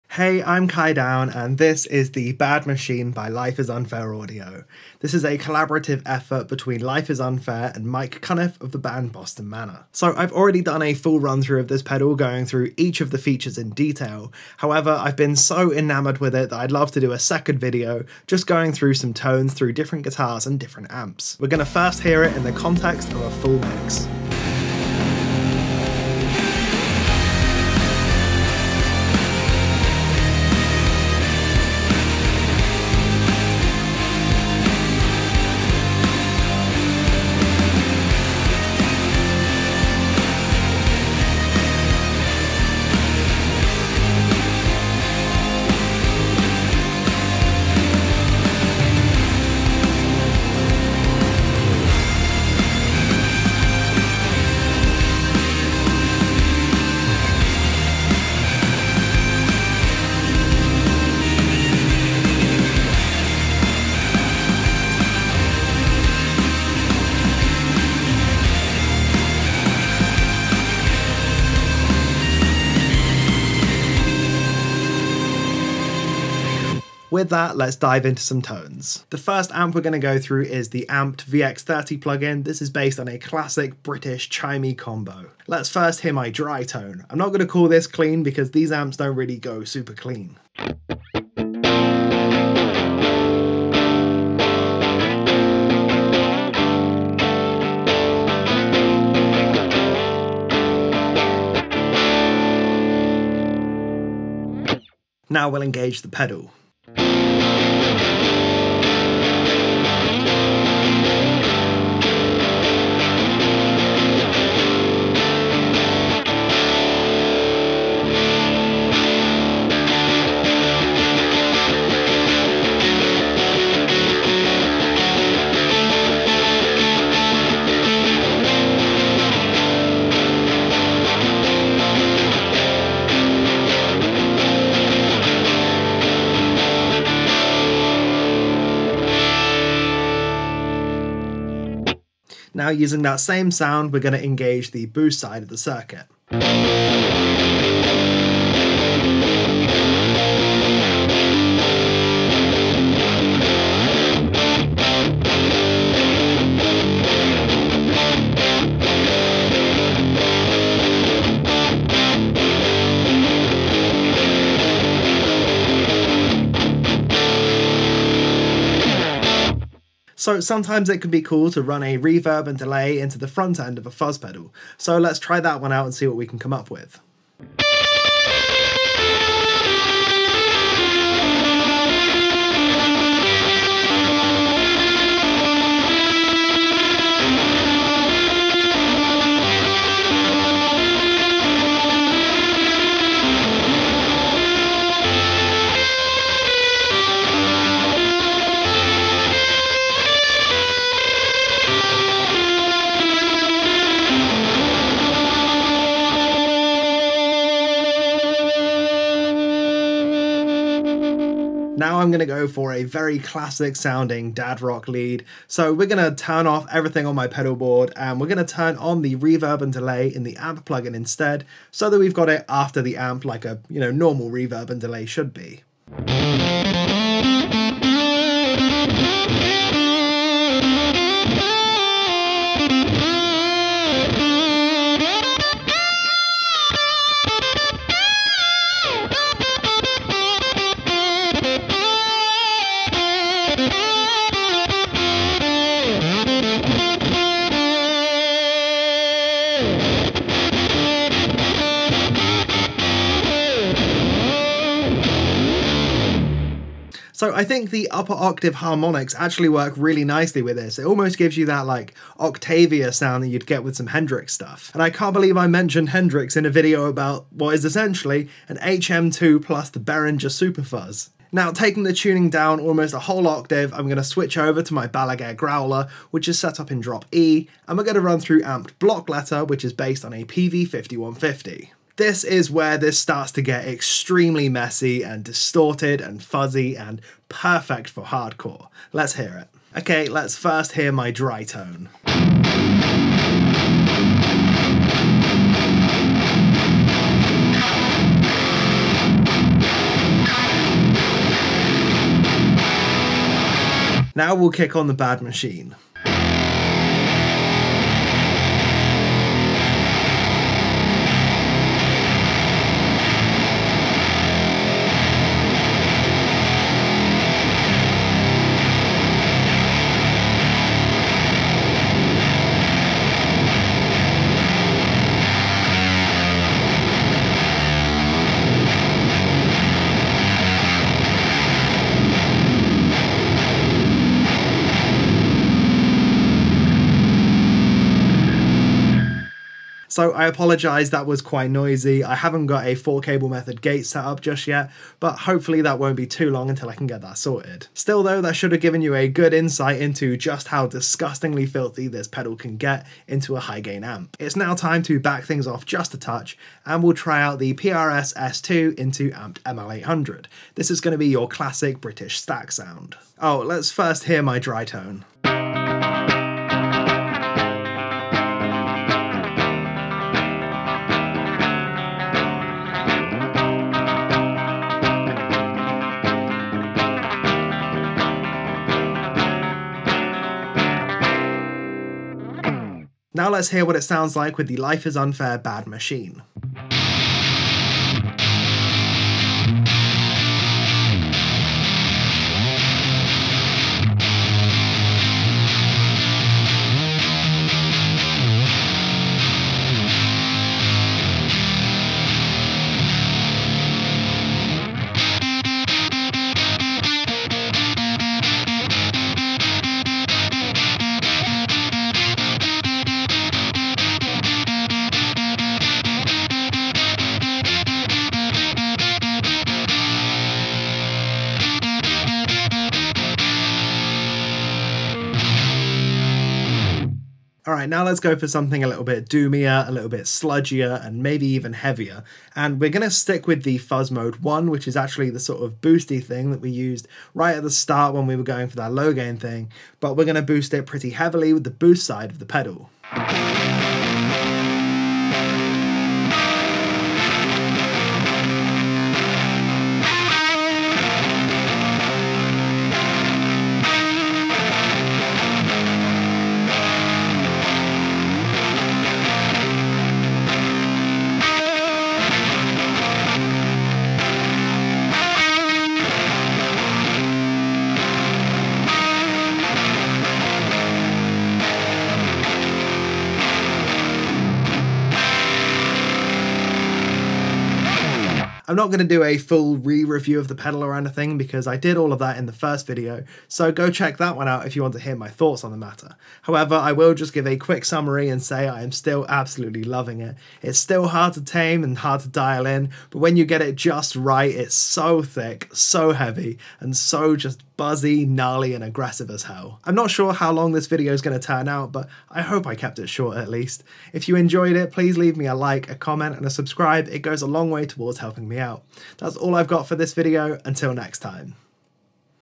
The Bad Machine Fuzz Is An Absolute BEAST | Sound Clips | Life Is Unfair Audio
Alright, I've already done a whole runthrough of this pedal, but I've been playing it so much recently that I couldn't help but come back to it. Decided to cut the waffle this time and pack as many sound clips in as I could.

Guitar Gear (Cables, Guitar Stands and Pedals)
Camera Rig (Cameras, Lenses, Lights and Stands)
Production Gear (Monitors, Interface, XLR)

TIMESTAMPS:
0:00 Intro
0:21 Full Mix Demo
1:19 Into Amped VX30 (British Combo)
4:49 Into Amped Block Letter (American High Gain)
5:54 Into Amped ML800 (British Stack)
7:58 Outro